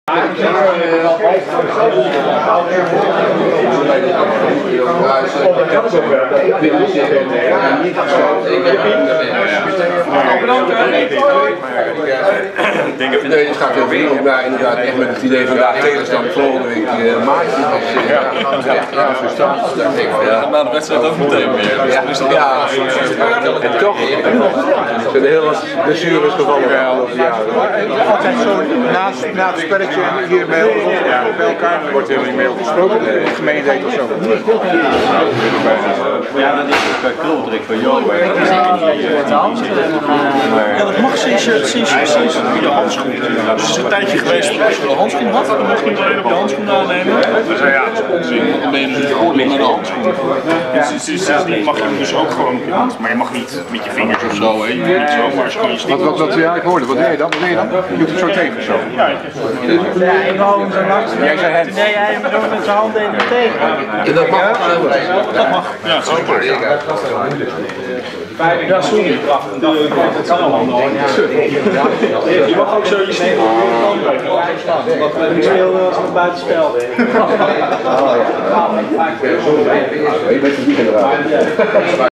de derde helft
Trimheren aan de koffie en thee. En een goed gesprek.